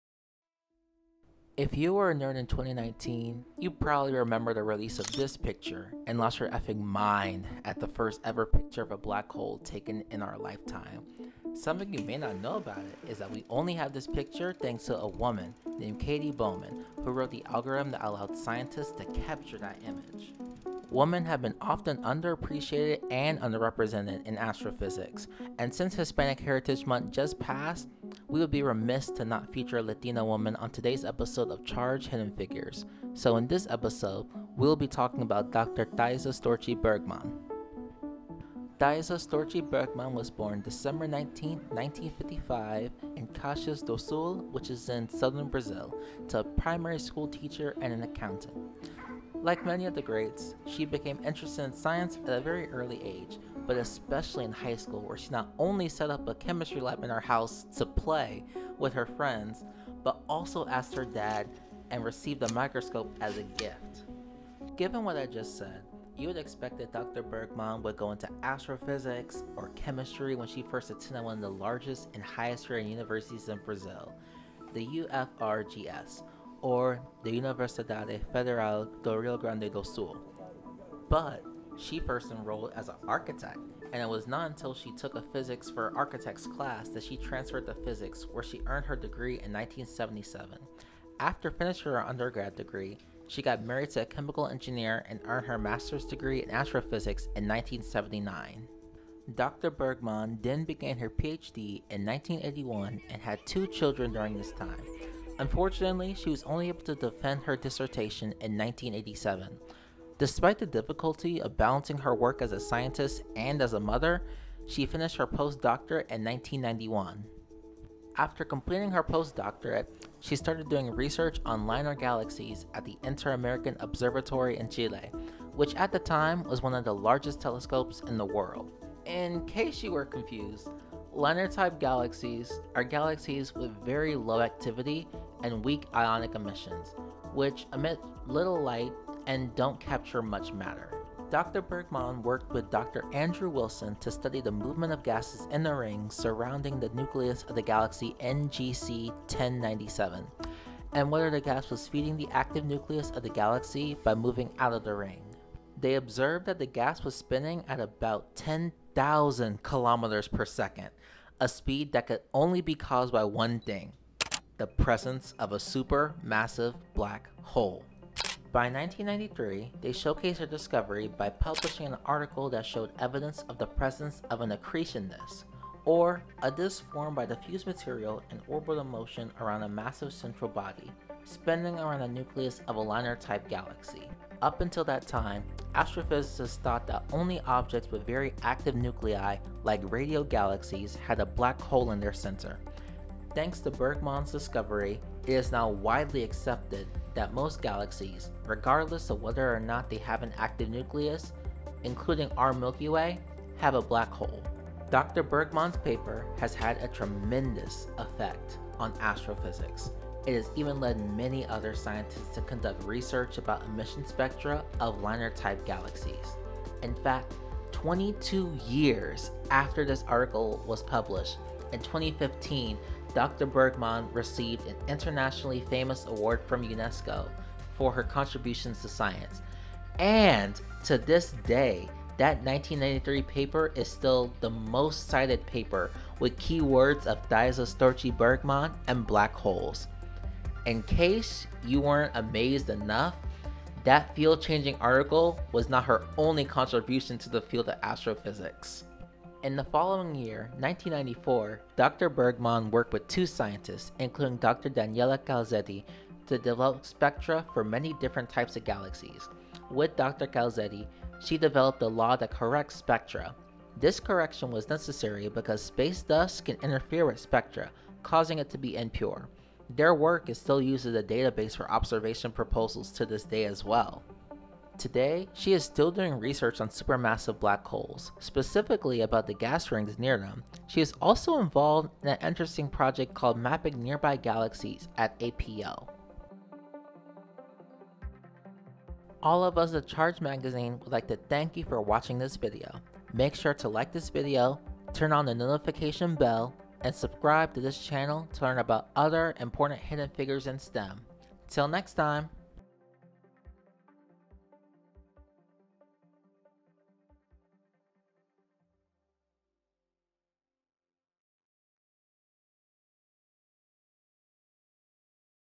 Hidden Figures: The Story of Thaisa Storchi Bergmann #1
Hi guys, welcome to our new channel! We are really happy you found us. This video is the first chapter to a series called Charged Magazine: Hidden Figures. Here we will feature amazing scientists and inventors in the STEM field forgotten throughout history. We hope you enjoy Dr. Storchi Bergmann as our first selection.